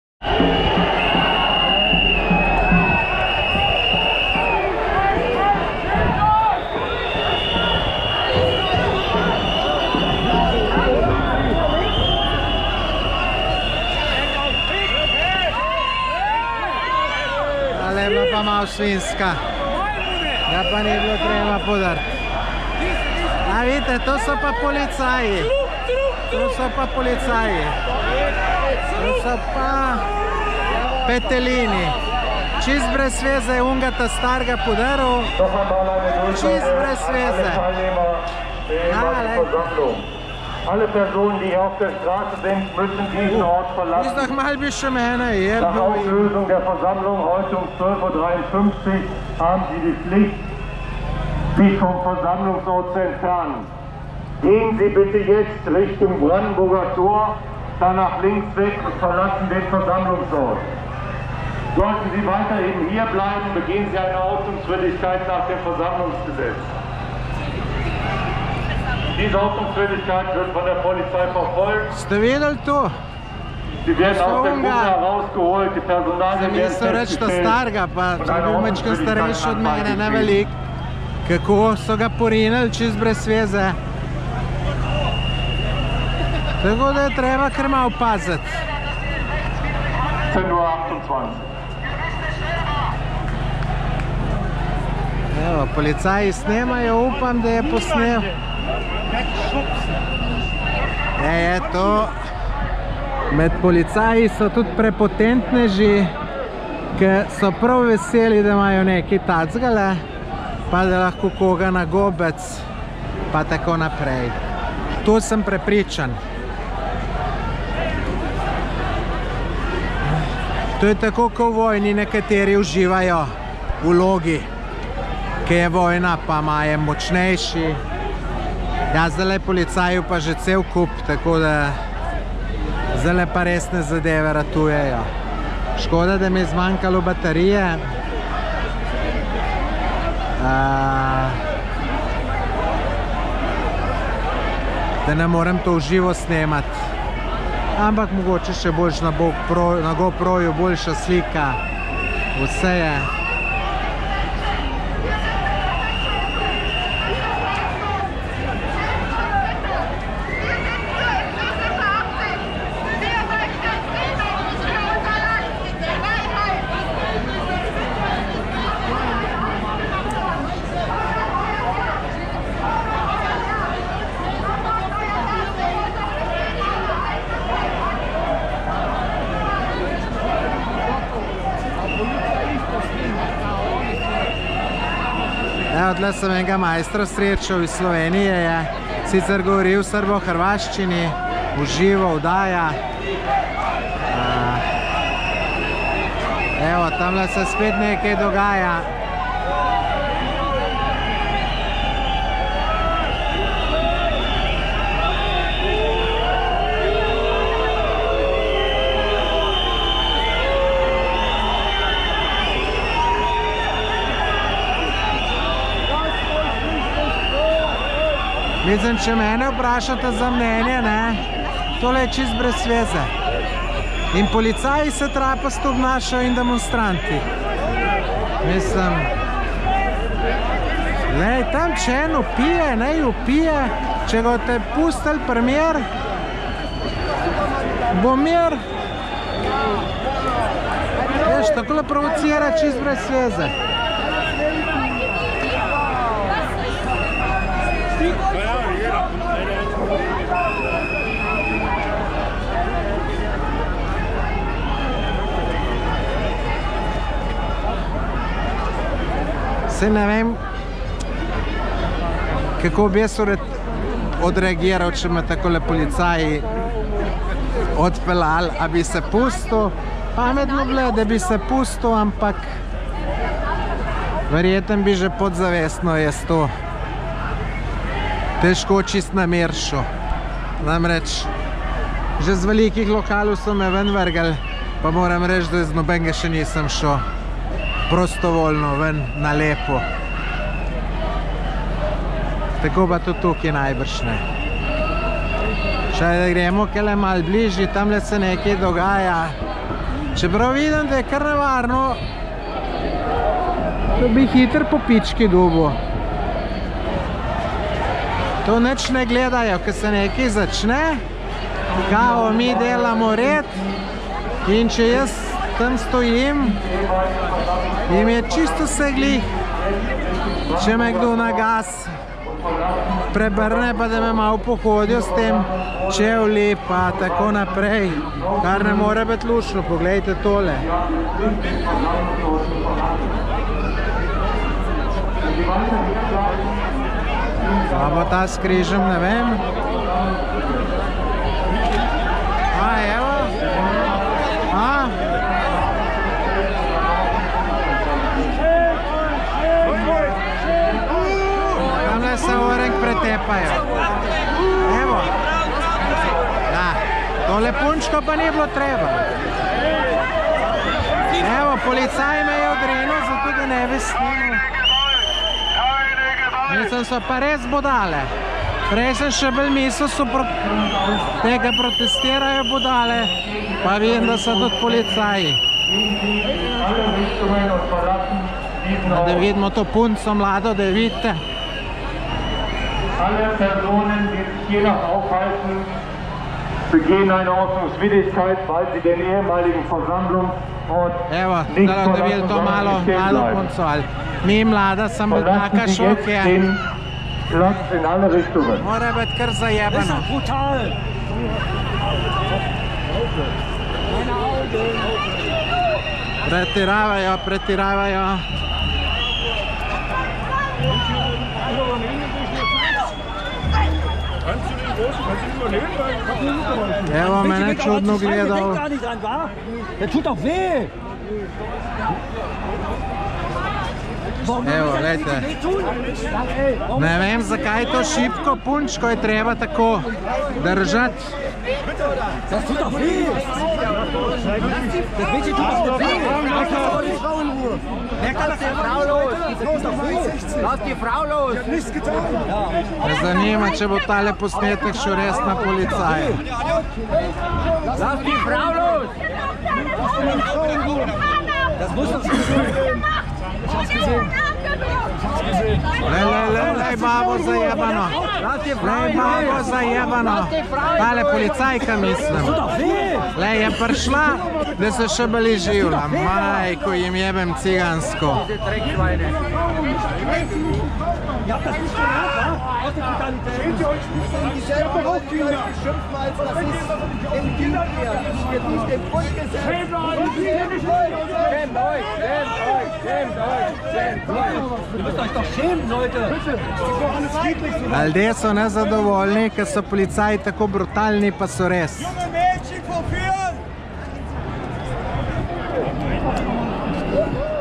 Policijsko nasilje na protestih v Berlinu
V soboto 29.8. sem obiskal Berlin, kjer so potekali protesti proti vladnim ukrepom glede koronavirusa.

V mojem video posnetku si poglejte, kako nasilni so bili policisti. Ni to edini video posnetek na katerem so policisti preveč agresivni,.. Te video posnetki še sledijo.